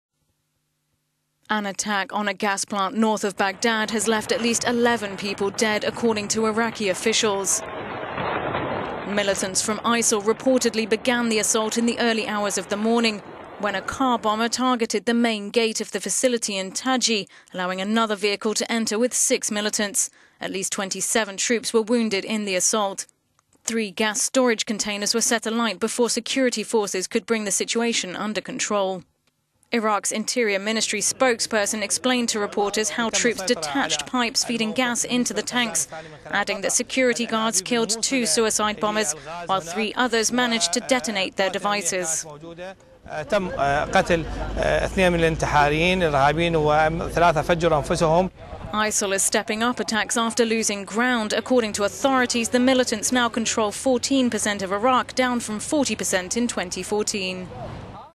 Eleven dead in attack on Iraq gas plant
An attack on a gas plant north of Baghdad has left 11 people dead, according to Iraqi officials.

Militants from ISIL reportedly began the assault in the early hours of the morning, when a car bomber targeted the main gate of the facility in Taji, allowing another vehicle to enter with six militants who launched the assault which left 27 troops wounded.

Three gas storage containers were set alight before security forces could bring the situation under control.